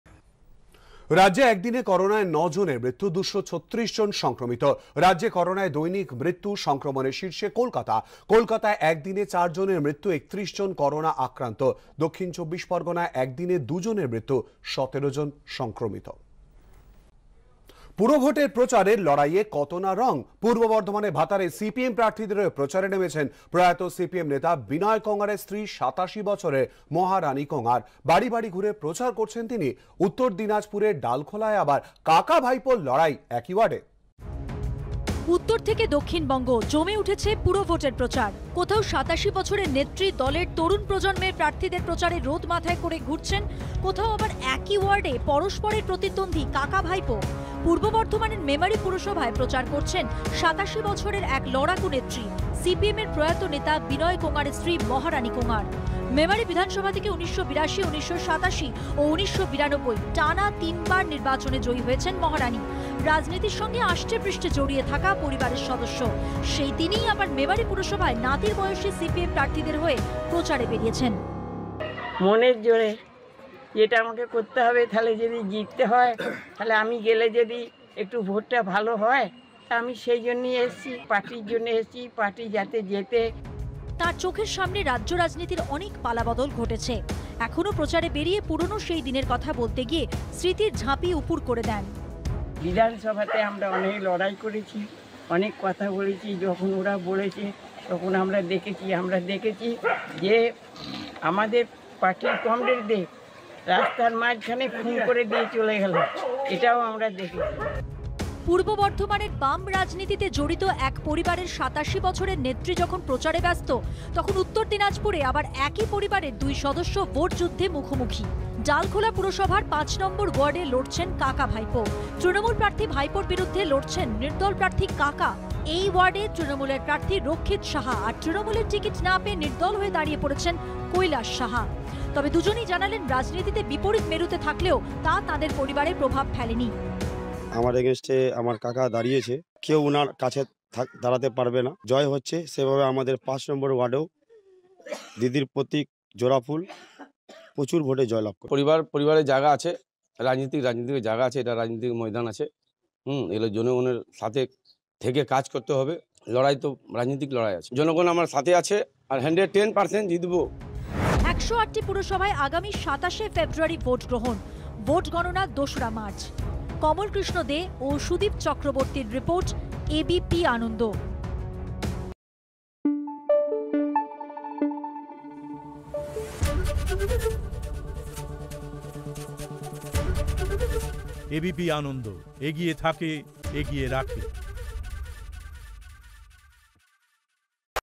State Corona: রাজ্যে করোনায় দৈনিক মৃত্যু, সংক্রমণে শীর্ষে কলকাতা। Bangla News
রাজ্যে একদিনে করোনায় ৯জনের মৃত্যু, ২৩৬জন সংক্রমিত। রাজ্যে করোনায় দৈনিক মৃত্যু, সংক্রমণে শীর্ষে কলকাতা। কলকাতায় একদিনে ৪জনের মৃত্যু, ৩১জন করোনা আক্রান্ত। দঃ ২৪ পরগনায় একদিনে ২জনের মৃত্যু, ১৭জন সংক্রমিত।